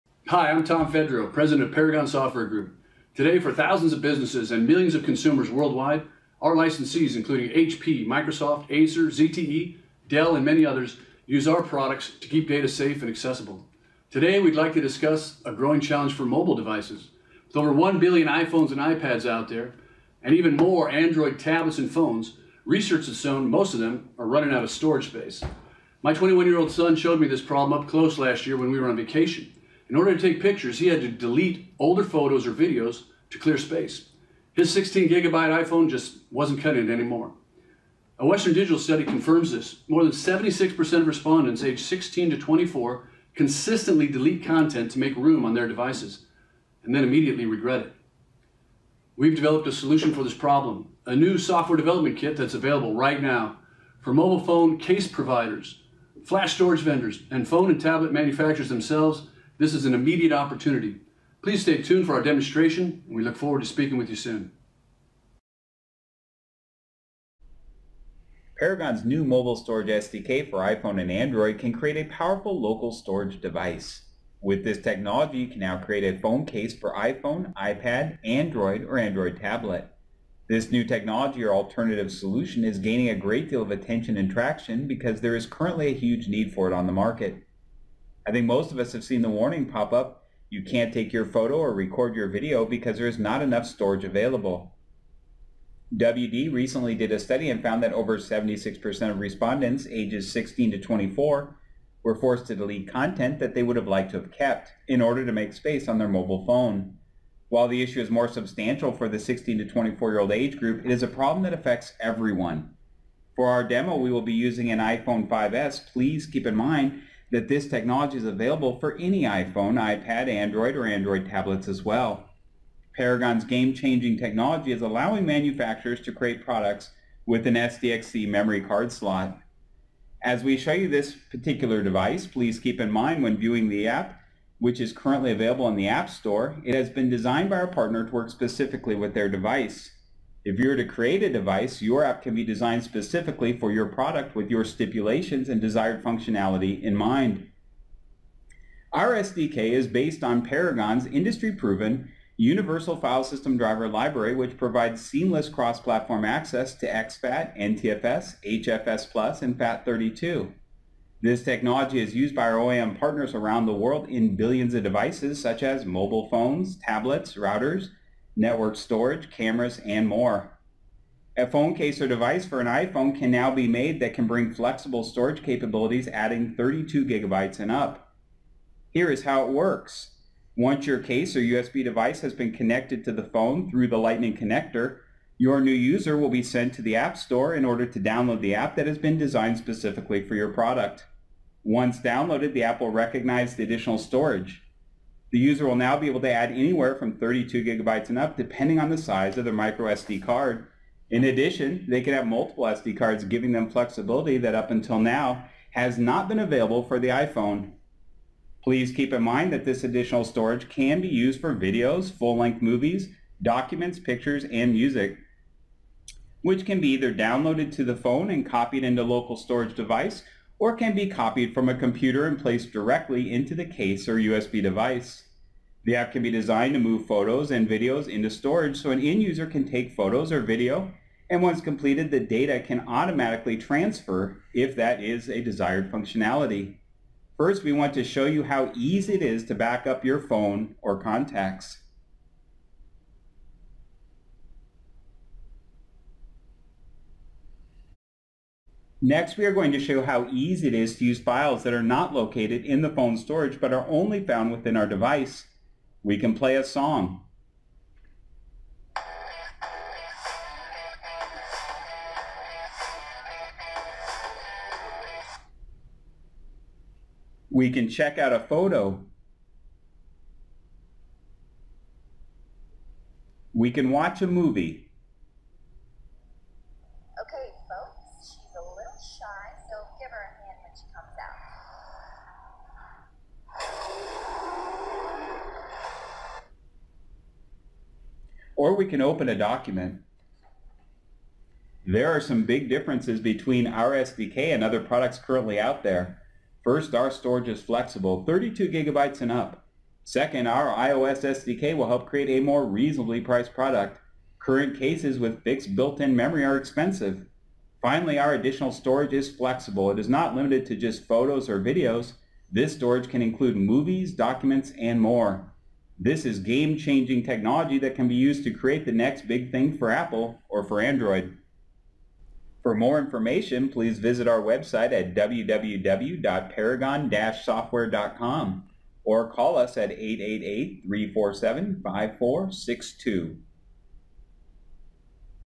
Expand Storage on your iOS Device
Paragon's new mobile storage SDK for iPhone can create a powerful 'local' storage device.  With this technology a manufacturer can create a phone case or other device (virtually any form factor) with the ability to expand an iPhone's storage capacity from 32 to GB to 1 TB.  This game changing technology is allowing manufacturers to create products with an SDXC memory card slot.